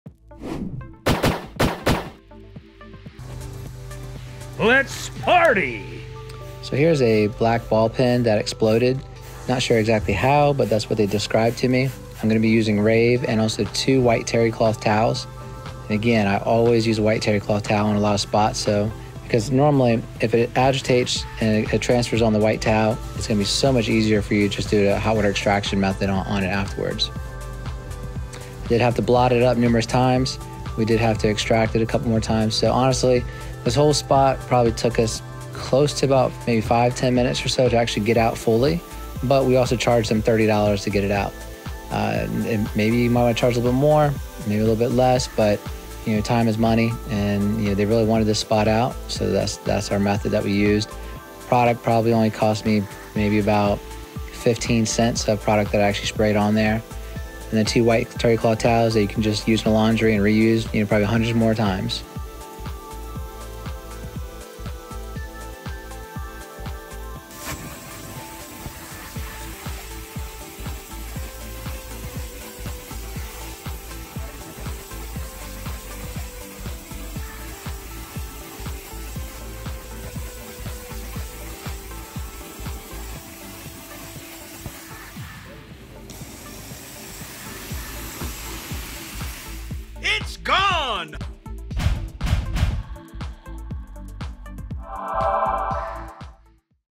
INK PEN EXPLODED On Carpet | How To Remove Ink From Carpet With Rave!
In this video, we show you how to get ink out of carpet using Rave! Ink pens are a common cause of ink stains in carpets, and this video will show you how to remove ink from carpets fast and easily! The best tip is to spray Rave Spotter (which we list below where you can get it from) and use white terry cloth towels to blot up, then extract using your portable or truckmount machine. Repeat if necessary the cleaning steps.

If you're a homeowner with ink stains in your carpet, this video is for you! We'll show you how to remove ink from carpet using Rave, a safe and effective method that's safe for the environment and your carpets.